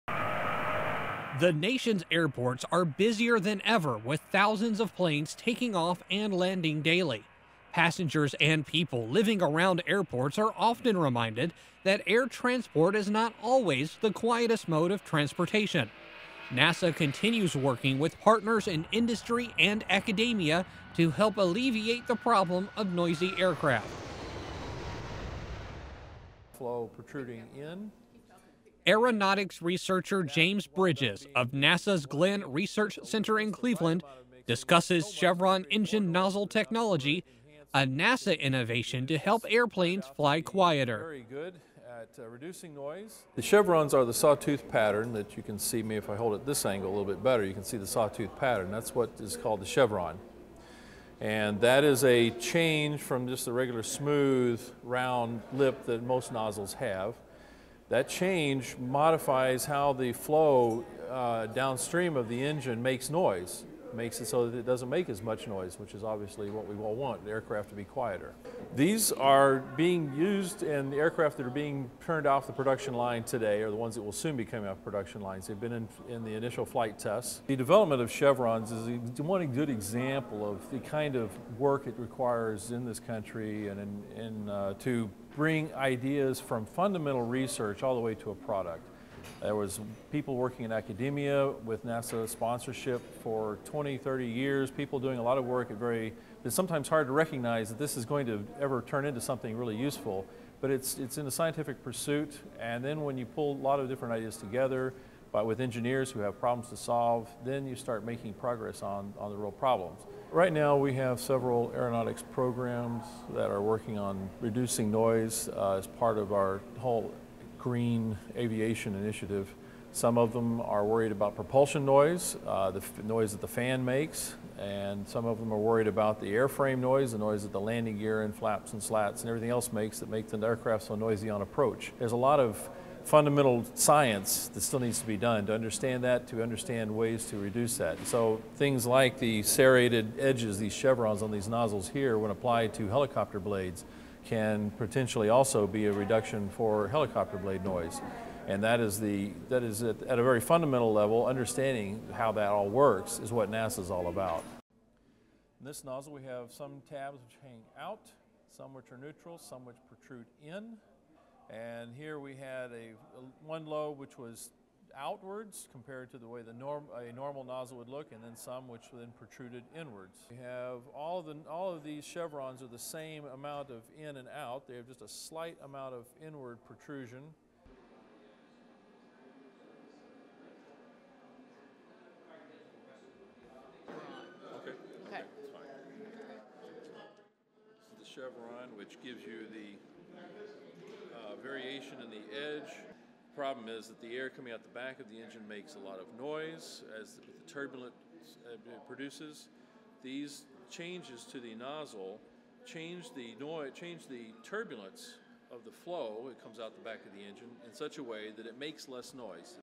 V Shaped Design Makes for Quieter Aircraft
Dr. James Bridges, aeronautics researcher at NASA's Glenn Research Center in Cleveland, describes how chevrons, one of the most recent noise-reducing technologies shepherded through the research process by NASA, is making commercial jet engines quieter.